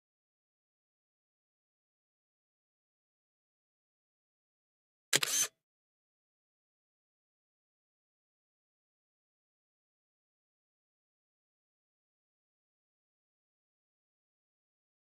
Lizard boy takes a selfie with dolphin man.
Lizard boy takes a selfie with dolphin man from life of luxury. This could be the start of an amazing friendship...